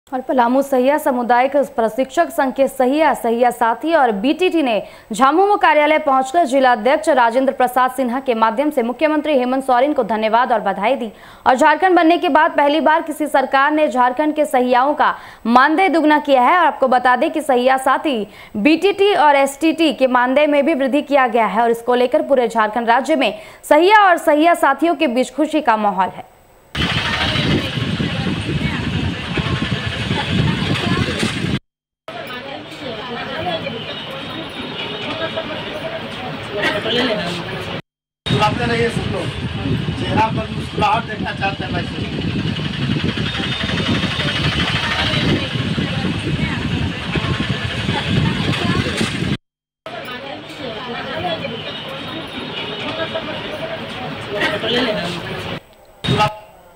पलामू : मानदेय में हुई वृध्दि को लेकर सहिया व सहिया साथियों में खुशी का माहौल
Raftaar Media